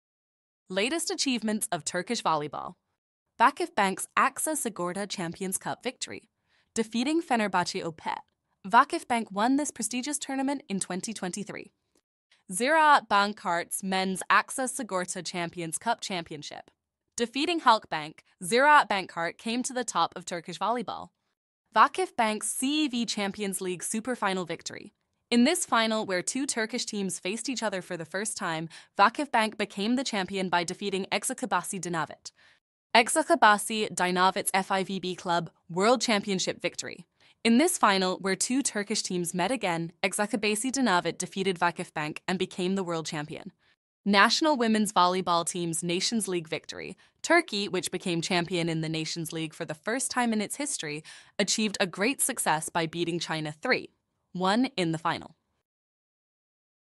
Latest Achievements of Turkish Woman Volleyball Team
Latest Achievements of Turkish Volleyball

VakıfBank's AXA Sigorta Champions Cup Victory: Defeating Fenerbahçe Opet, VakıfBank won this prestigious tournament in 2023.

Ziraat Bankkart's Men's AXA Sigorta Champions Cup Championship: Defeating Halkbank, Ziraat Bankkart came to the top of Turkish volleyball.

VakıfBank's CEV Champions League Super Final Victory: In this final where two Turkish teams faced each other for the first time, VakıfBank became the champion by defeating Eczacıbaşı Dynavit.

Eczacıbaşı Dynavit's FIVB Club World Championship Victory: In this final where two Turkish teams met again, Eczacıbaşı Dynavit defeated VakıfBank and became the world champion.

National Women's Volleyball Team's Nations League Victory: Turkey, which became champion in the Nations League for the first time in its history, achieved a great success by beating China 3-1 in the final.